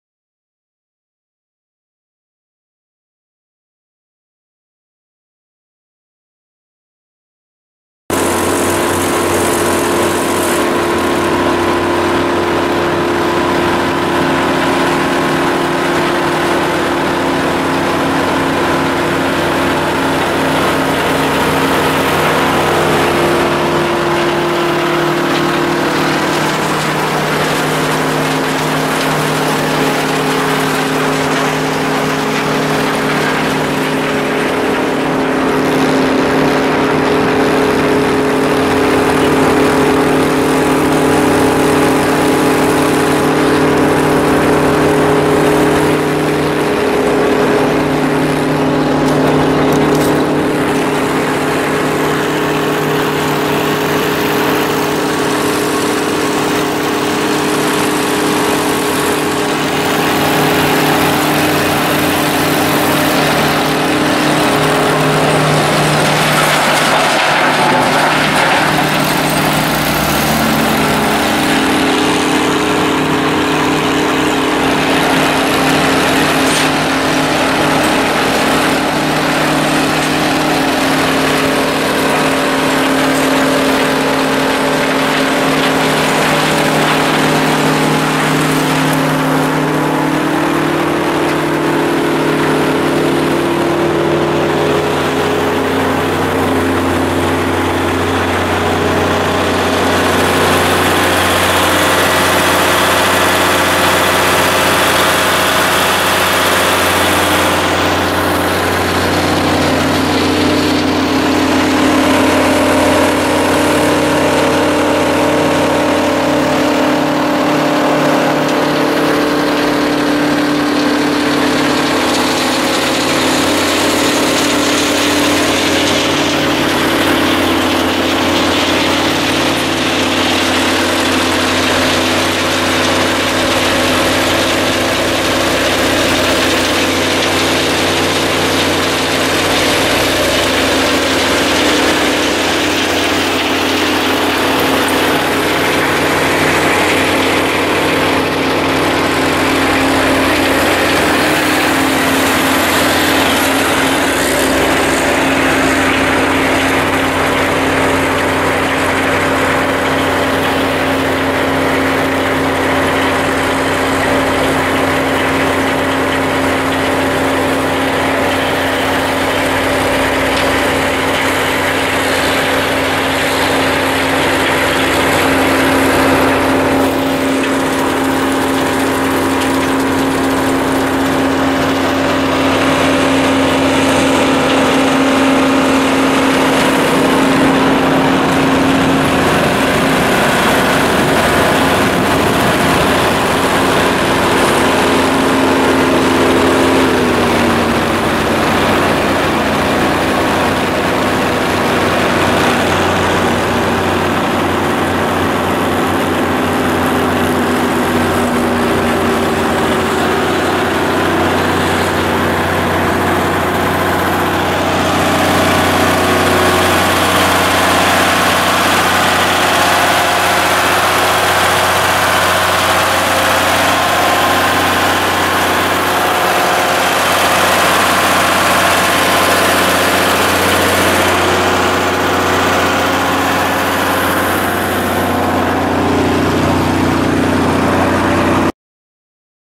1966 Missouri Rogers rock impact crusher for sale | sold at auction June 26, 2014
1966 Missouri Rogers rock impact crusher
Detroit Diesel 8V71 diesel engine
43.5&#039;L x 114"W x 14&#039;H
38" x 50" horizontal shaft impact crusher
34&#039; x 35" front discharge conveyor
SECO vibratory grizzley feeder
30 HP electric motor
Fifth wheel
Air brakes
10.00-20 tires
Machine has been been dis-assembled and ready for transport
Additional trailer will be needed to haul all parts and blocking with crusher
Serial 3850-PDF-PP-229

It was located in Creston, WY, and we sold it to the highest bidder for $18,700.